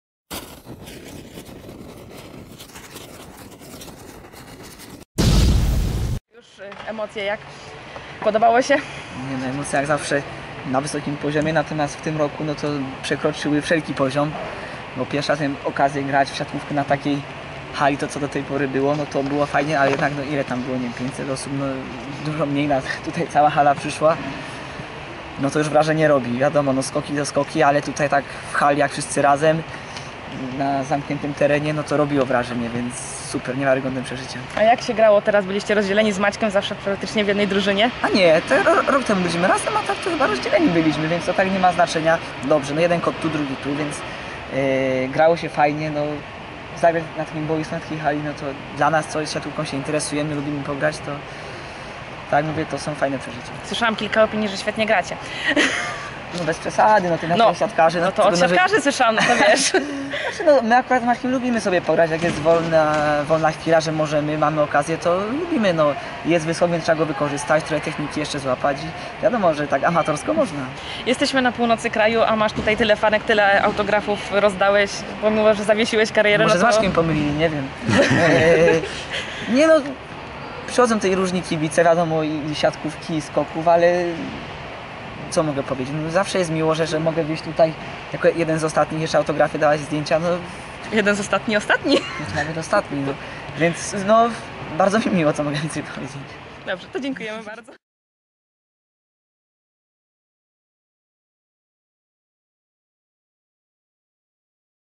Mecz Gwiazd Szczecin 2015: wywiad z Jakubem Kotem [Skoki24.pl]
Rozmowa z Jakubem Kotem przeprowadzona zaraz po Meczu Gwiazd, który odbył się 30 maja w Szczecinie.

Wideo: Karolina Koster, Kacper Kolenda
Skoki24.pl